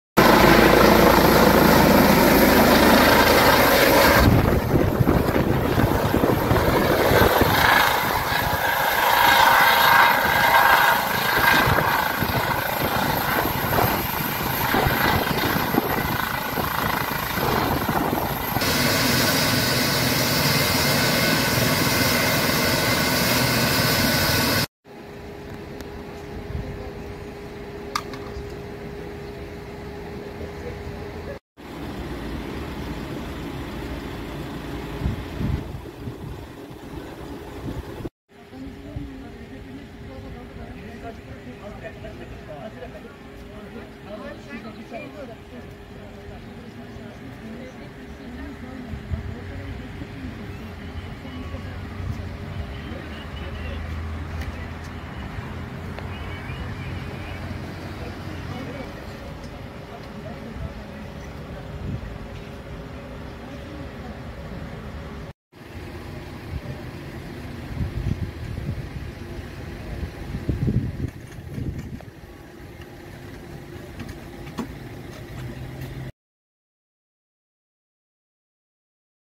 Evde baygın bulunan kadın ambulans helikopterle hastaneye sevk edildi
Samsun'da evde baygın bulunan kadın, ambulans helikopterle hastaneye sevk edildi.